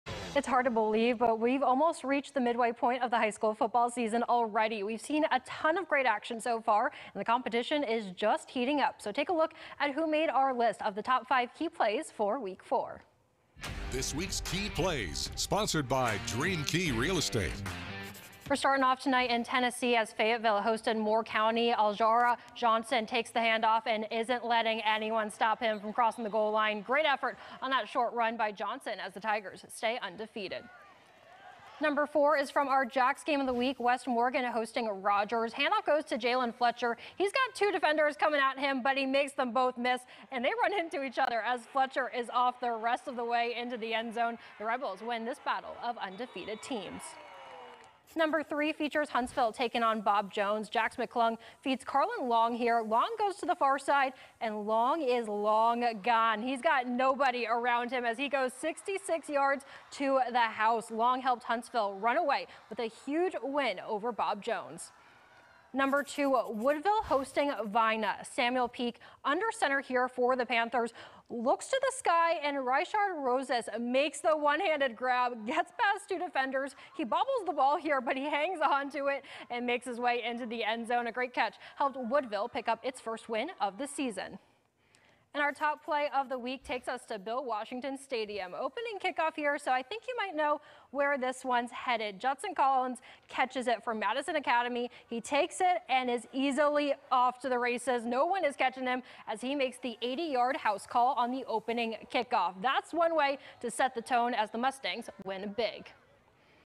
Week 4: Top Five Key Plays of the Week
The fourth week of high school football season is in the books which means we saw a ton of incredible plays on the gridiron from our Tennessee Valley teams.